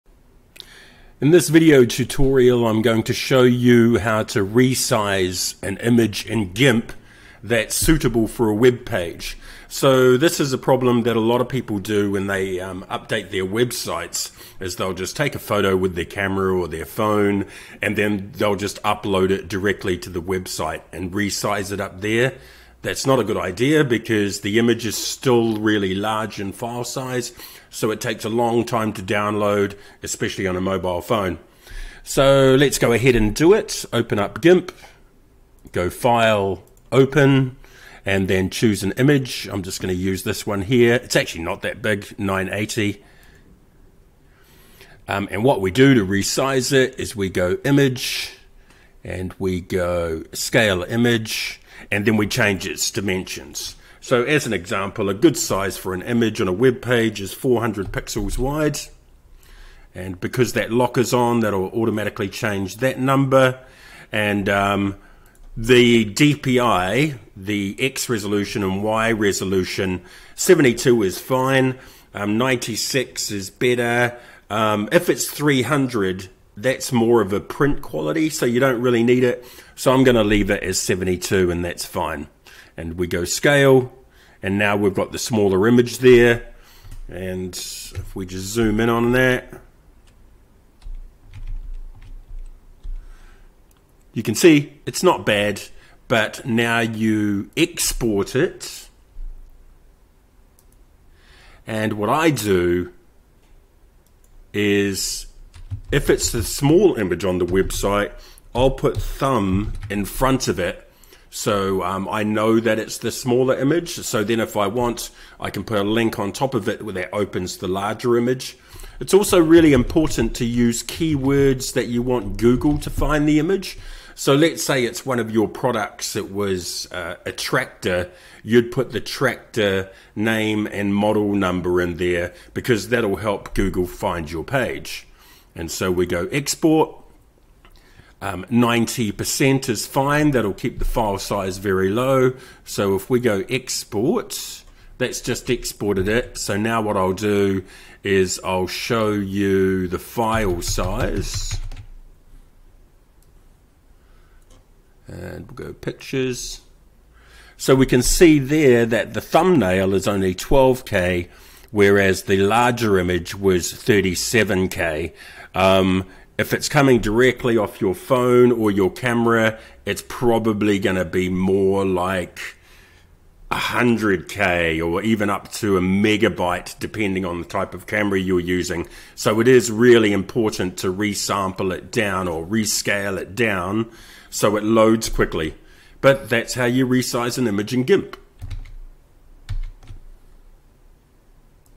Resize Image Using GIMP (2019)
In this tutorial I show you how to quickly resize an image in the free GNU Image Manipulation Program (GIMP) available If you'd like to learn 3 ways to crop an image, view this video